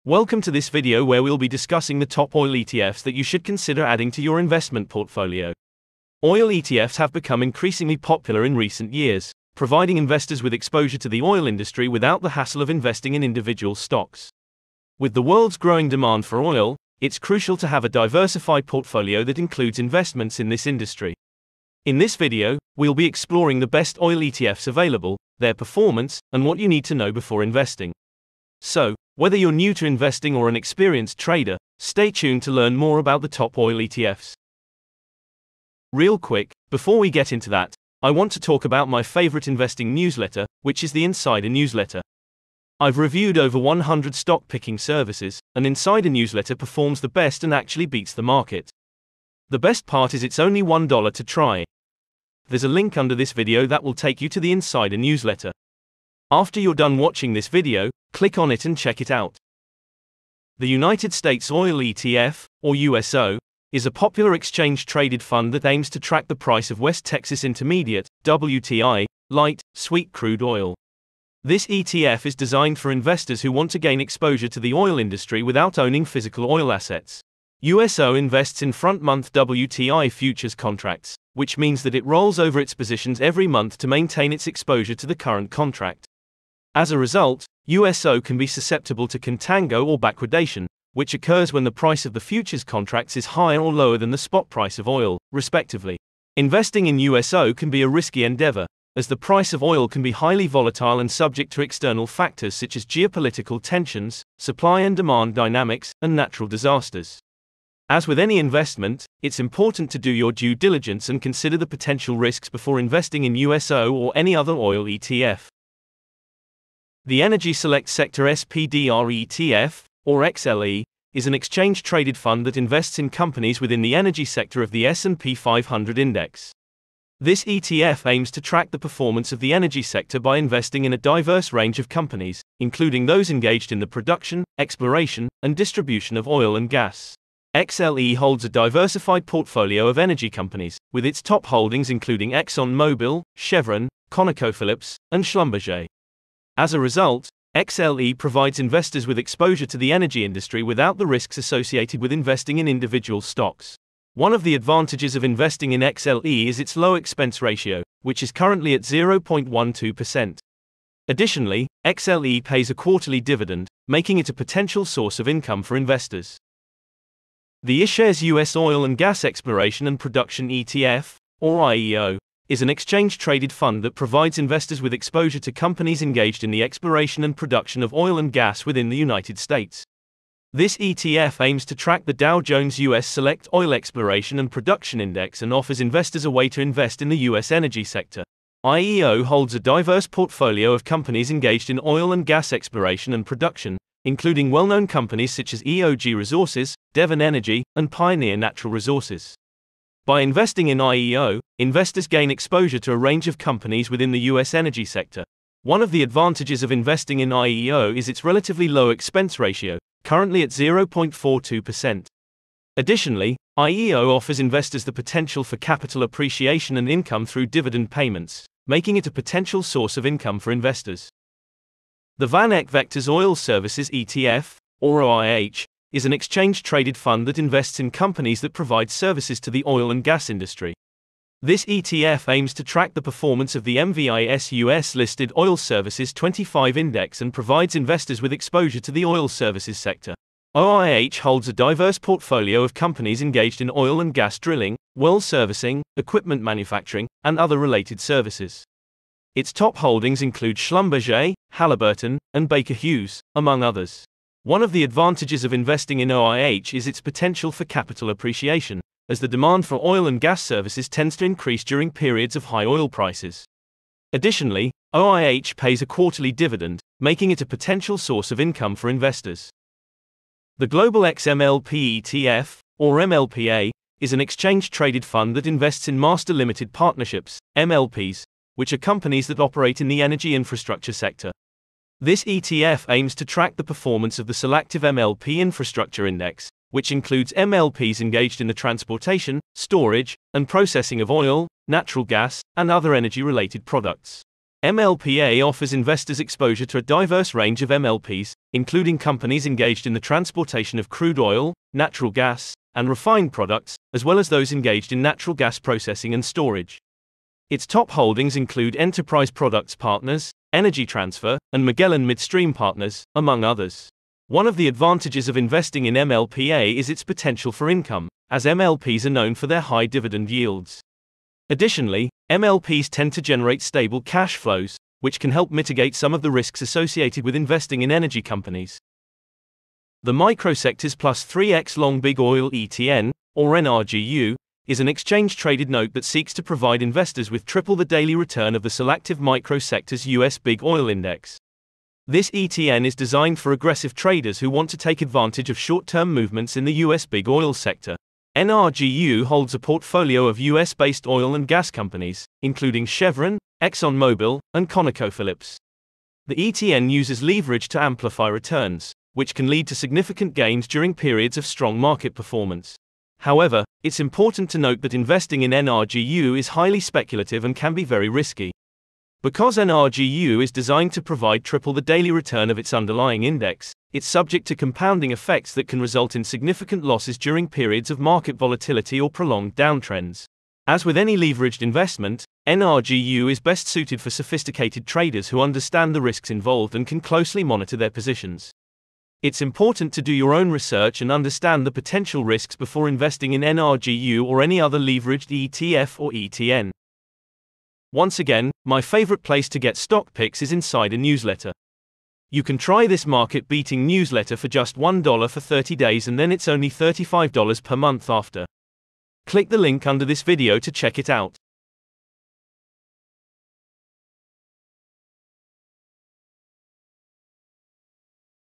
6 Best Oil ETFs To Buy Right Now
In this video, we'll be discussing some of the top oil ETFs available in the market today. Oil ETFs have become increasingly popular among investors who want to gain exposure to the oil industry without investing in individual stocks or owning physical oil assets.

We'll cover the United States Oil ETF (USO), which tracks the price of West Texas Intermediate (WTI) light, sweet crude oil, and the Energy Select Sector SPDR ETF (XLE), which invests in companies within the energy sector of the S&P 500 Index.

We'll also discuss the iShares US Oil & Gas Exploration & Production ETF (IEO), which provides investors with exposure to US-based companies engaged in the exploration and production of oil and gas, and the VanEck Vectors Oil Services ETF (OIH), which invests in companies that provide services to the oil and gas industry.

Lastly, we'll touch on the Microsectors +3X Long Big Oil ETN (NRGU), which seeks to provide investors with triple the daily return of the Solactive MicroSectors US Big Oil Index. We'll highlight the potential benefits and risks of investing in these ETFs and ETN, and provide tips on how to evaluate which oil ETF may be the right fit for your investment goals and risk tolerance.